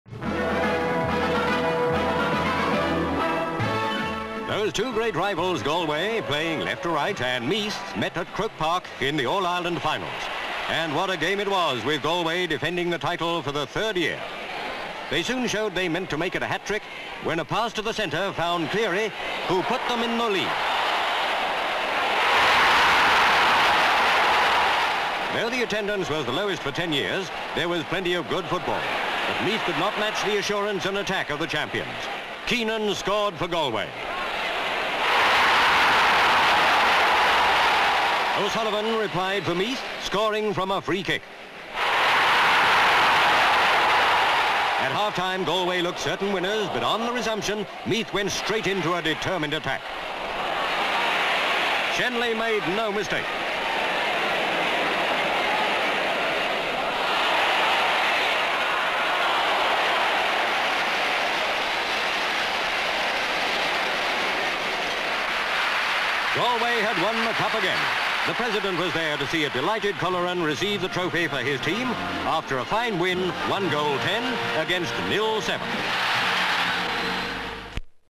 Galway Win Gaelic Final (1966)
Dublin, Republic of Ireland (Southern Ireland, Eire).

Start of the Gaelic Football Cup final between Galway and Meath in Dublin. GV. The game is started, Galway kicking left to right LV. As Galway retains possession. The ball is passed to the middle and Cleary scores a point. SV. Crowd. LV. General play. GV. Keenan gets possession for Galway and scores another point. CU. Crowd cheering. GV. O'Sullivan scores a point from a free kick for Meath. GV. Crowd at half time. SV. The game is restarted for second half and Meath start an attack. GV. Shanley scores a point for Meath by punching ball over bar. GV. Pan, Galway attacking. LV. As Galway forward tries a shot which is saved by McCormack and is bundled over the line by Galway forward. GV. Crowd cheering at the end of game. Galway wins the final. CU. President of Ireland Eamonn de Valera. SV. Galways captain Colleran receives cup and holds it aloft.
 FILM ID:2009.06